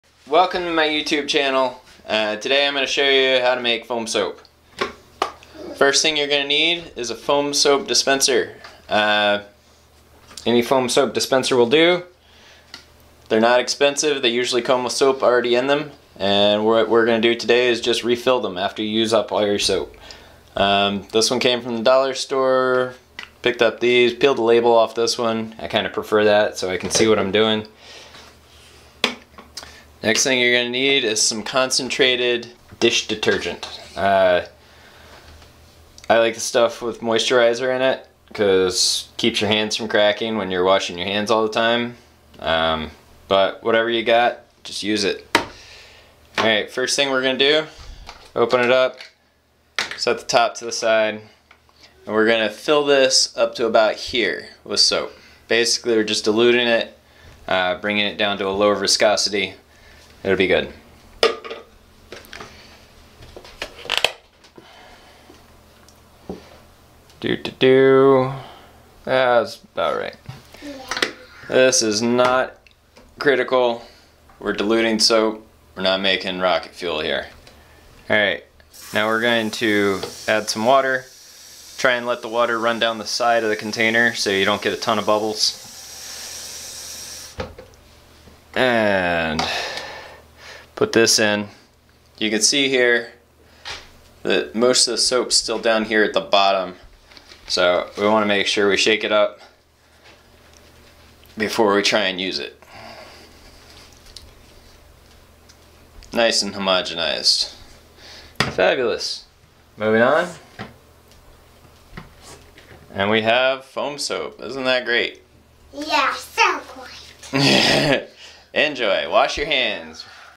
Making Your Own Foam Soap
I have been making my own foam soap for years.  Now that we are in the midst of a pandemic, everyone should be washing their hands regularly, but with cleaning supplies being limited, I thought it time make a short video showing how to take a little bit of dish soap, and turn it into a lot of hand soap.  One big bottle of dish soap like I show in the video is enough to last my family at least six months of regular hand washing.  There are certainly other people who do this, but we should all be sharing information freely in this important time in our history, so pass it around and be safe!